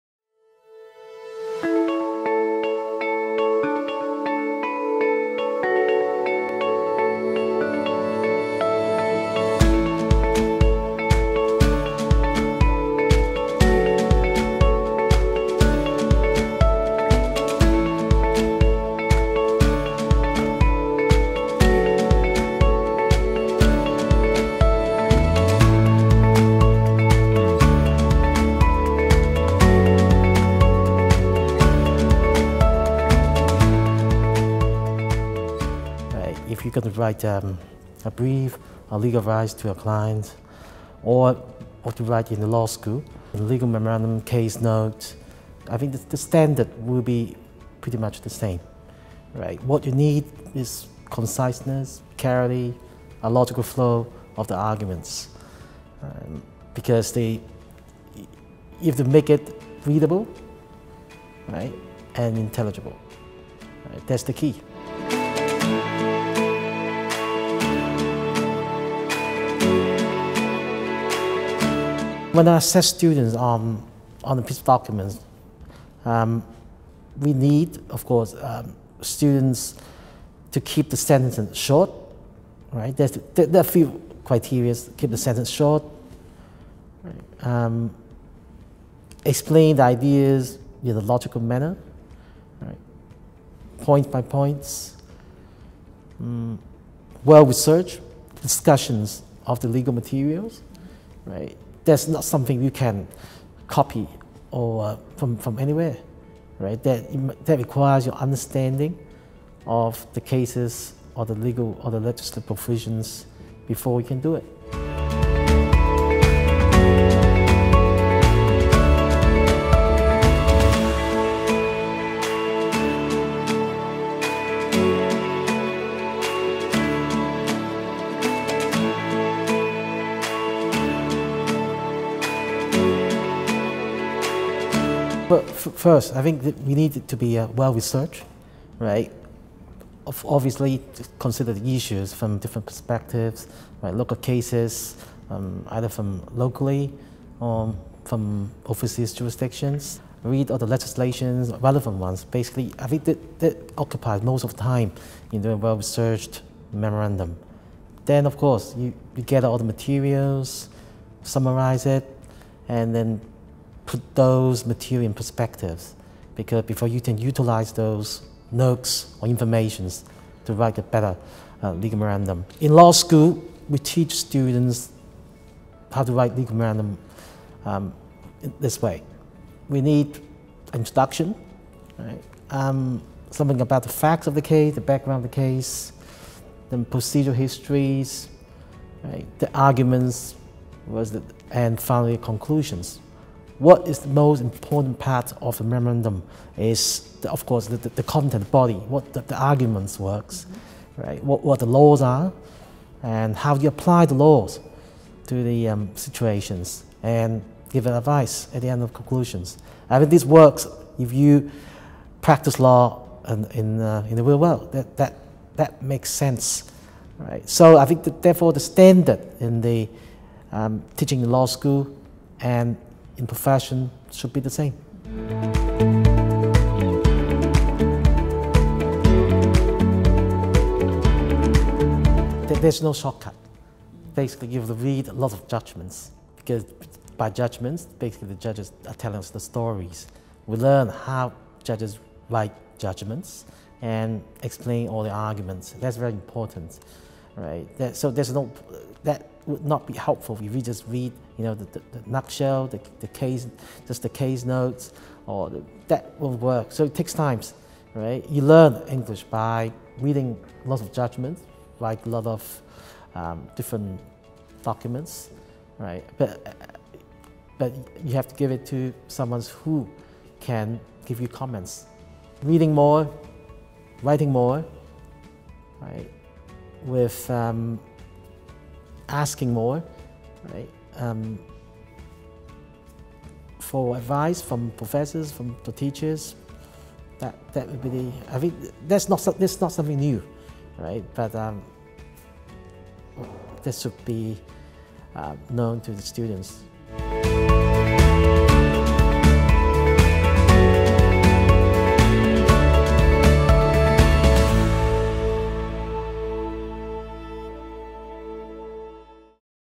Mr Alexander Mak (1): Advice on legal writing
An interview with Mr Alexander Mak featuring his advice on legal writing as a legal educator as well as a legal professional. Includes advice to law students. Produced by legal English in Hong Kong, with support from the Hong Kong University Grants Committee.